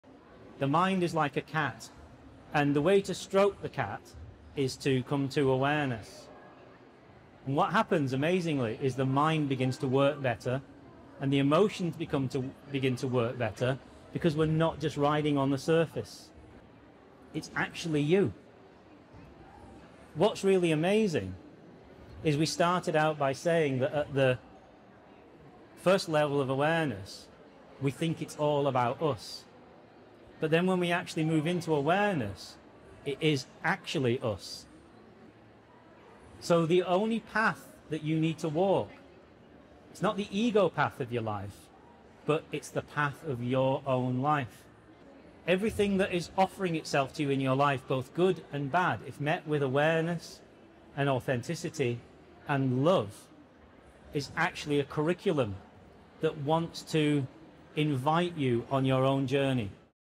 ‘Awareness’ - A snippet from Alexandra Palace Talk
Snippet from my talk in awareness at mind body soul show recently. Whole talk will be shared soon. Like and follow to stay in the loop